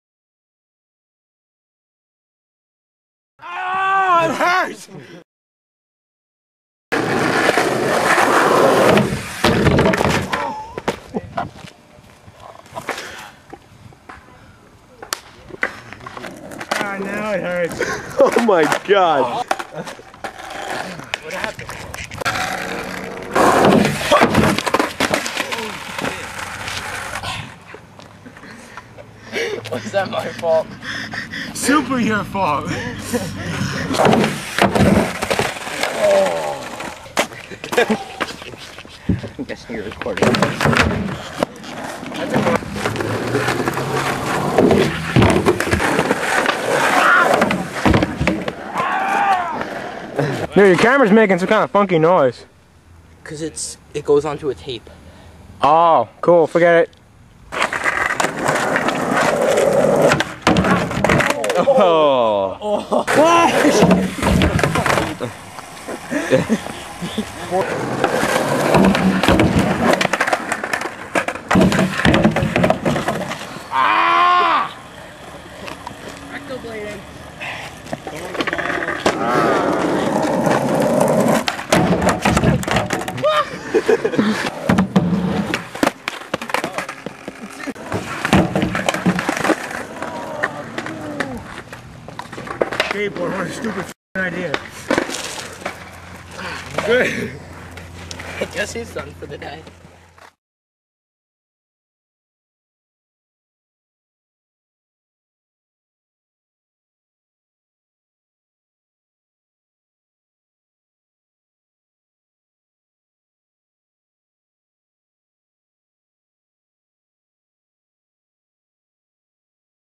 Brian Searight Bails
All in one day, Brian Searight can take a few good falls. Heres some, and dont worry, he makes more than he misses.     not in this video.